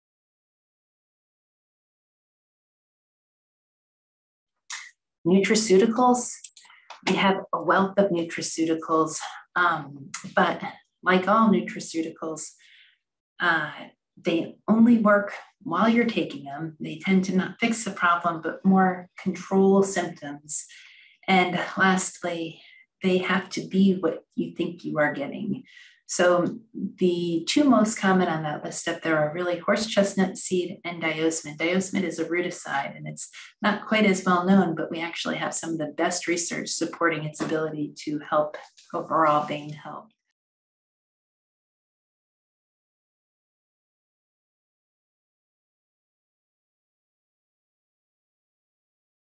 Nutraceuticals in Vein Care
At the Vein Healthcare Center, Dr. Cindy Asbjornsen has made a difference in the life of seniors at every stage – young old, old, and old old. In this session she will describe what happens to veins as they age and then outline the treatments that are available to patients.

Many seniors think varicose veins and venous ulcers are a normal part of aging, but what they may not know is that they need not suffer with symptoms. Vein disease will slow down older adults. They may do less walking and have less mobility in general, which might make them less likely to connect with the outside world. The good news is it is treatable, often with excellent outcomes and huge improvements in quality of life.